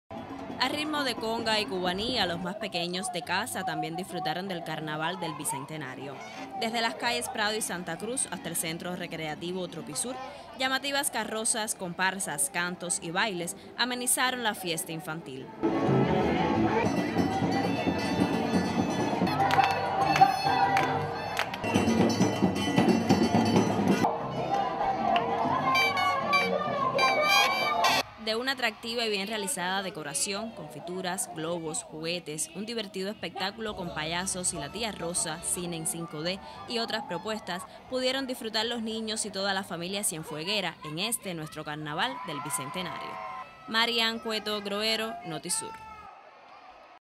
Celebran carnaval infantil en Cienfuegos
Bajo la dirección artística de Miriam Santander y la casa de la Cultura, fue realizado el carnaval infantil, con la participación de 5 carrozas, comparsas, muñecones y otras propuestas que disfrutaron toda la familia cienfueguera.